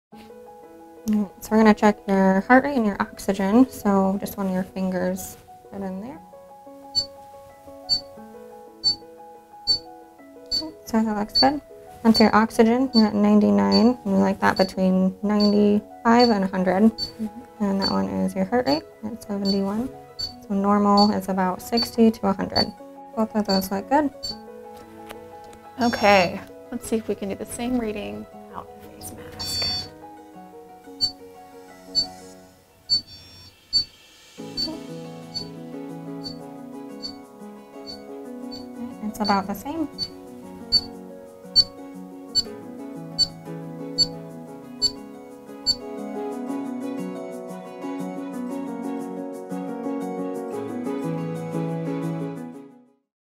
Does Wearing a Face Mask Actually Affect Our Ability to Breathe?
Does wearing a mask affect your breathing? With the help of LPN Suzanne Sundbald, Sanford Health News host Courtney Collen tests the difference in her pulse and oxygen levels with and without a face mask.